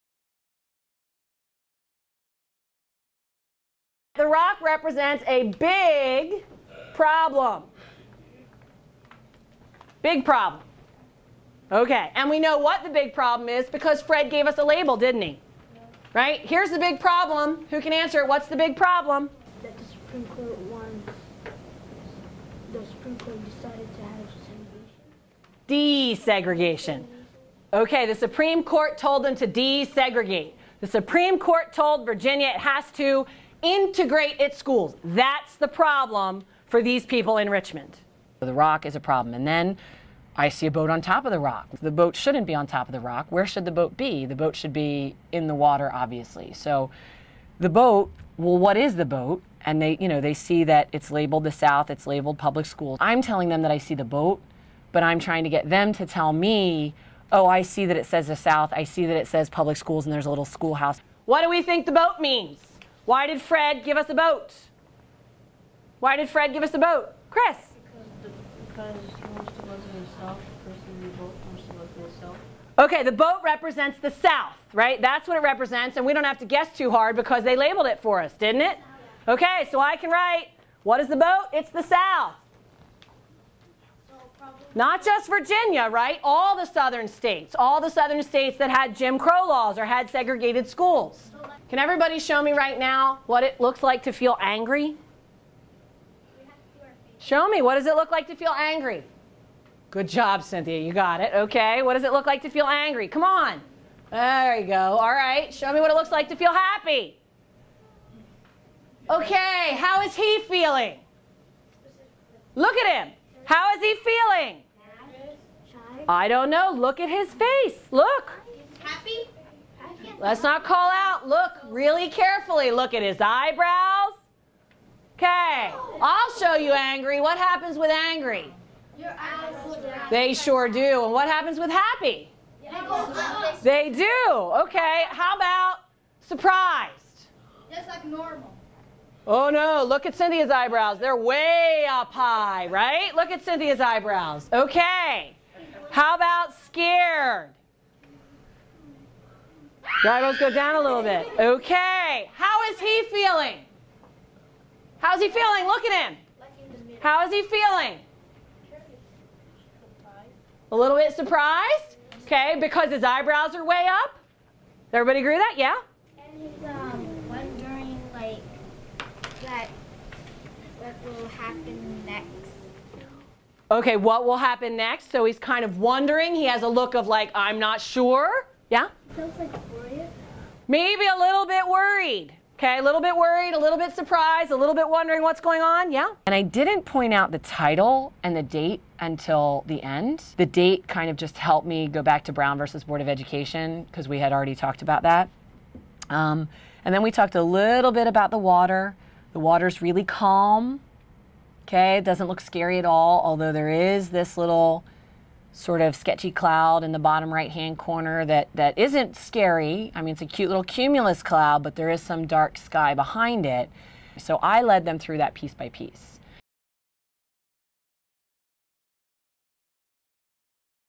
Teaching in Action - Massive Resistance - Identifying the Parts of a Cartoon (Con't)
Stacy Hoeflich's 4th-grade students learn about Virginia's policy of massive resistance to public school desegregation by carefully reading and examining two political cartoons.